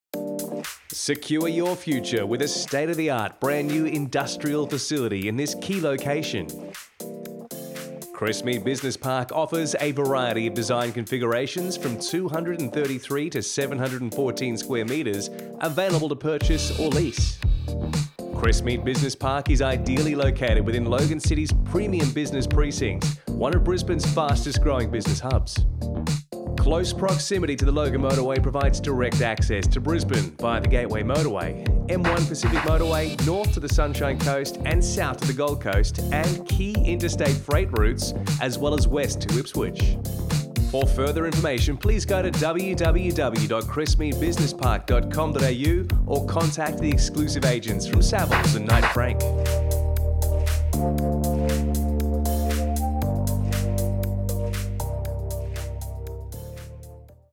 Crestmead Business Park, QLD
Savills are proud to be entrusted with the sale and leasing of Crestmead Business Park. a premium business precinct with 29 units available. configurations are available from 233-714 sqm*
In conjunction with Knight Frank.